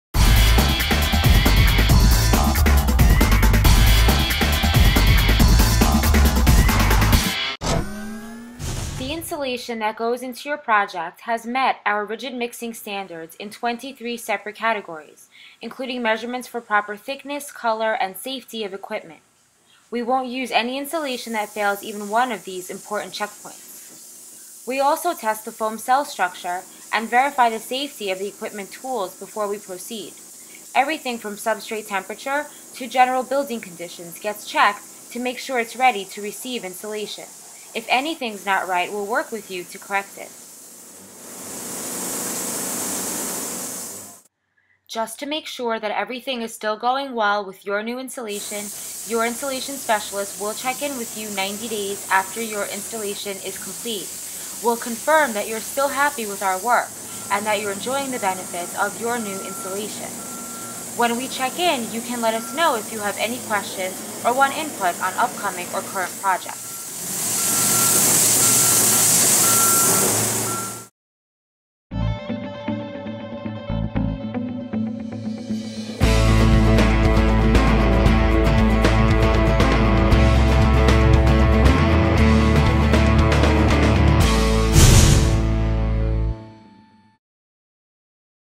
Spray Foam Insulation - Parking Garage Spray Foam Insulation Applic.m4v
- Thinking of using spray foam insulation? Learn how open cell polyurethane foam insulation was used in a residential parking garage in this short video.

Coastal Insulation serves New Jersey, New York, Connecticut, and Pennsylvania.